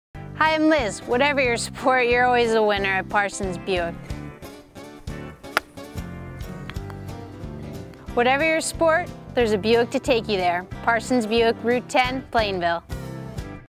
Your Always A Winner At Parsons Buick
LPGA pro Liz Janangelo lets us know, What ever your sport you always a winner at Parsons Buick of Plainville. Check out this golf trick shot. Centrally Located Rt. 10 Plainville.